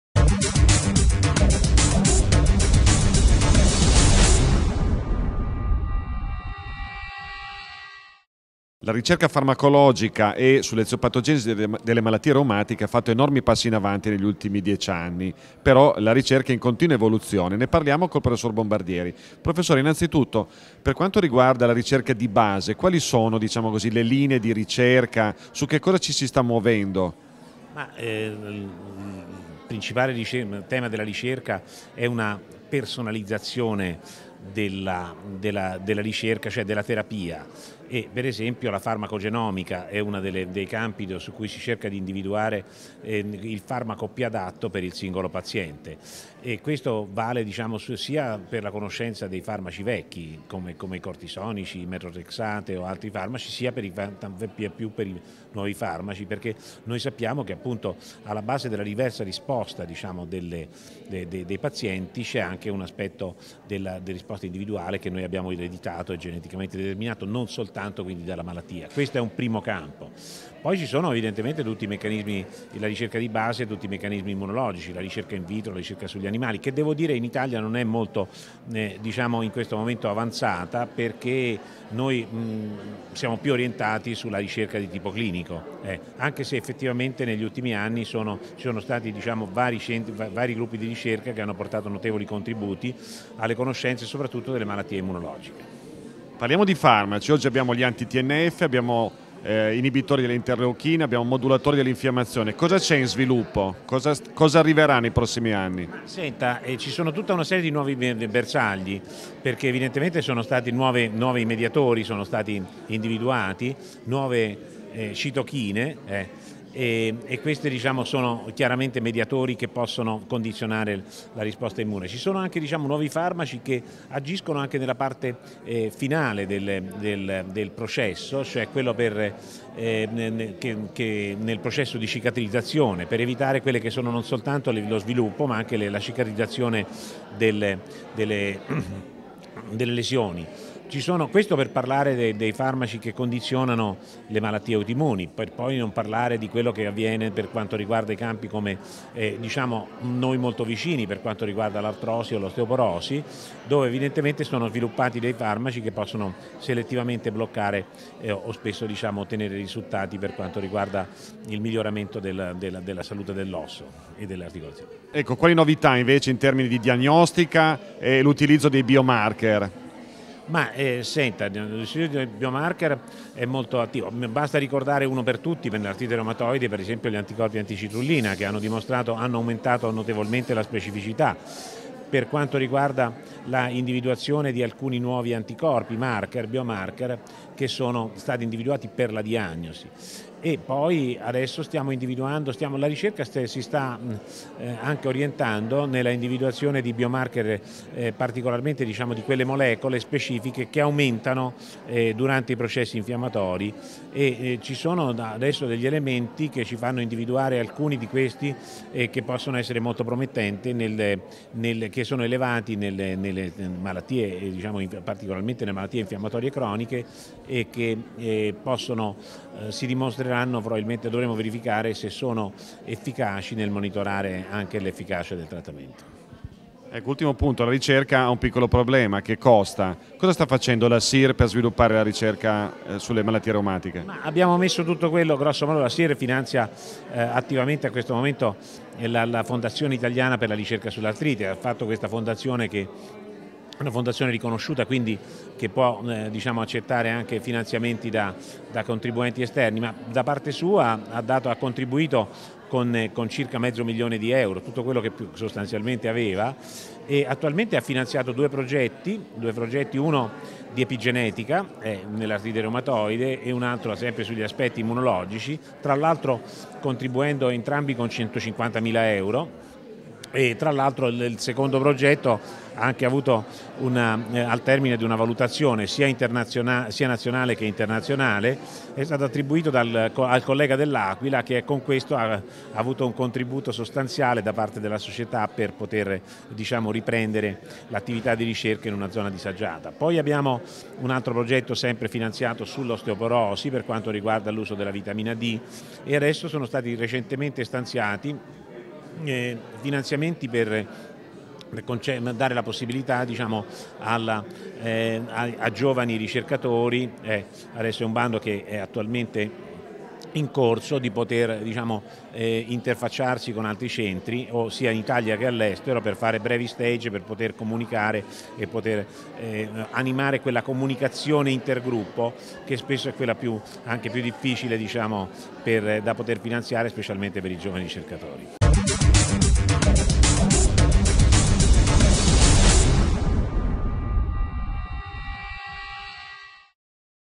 Attività di FIRA per la ricerca in reumatologia
Intervista al prof. Stefano Bombardieri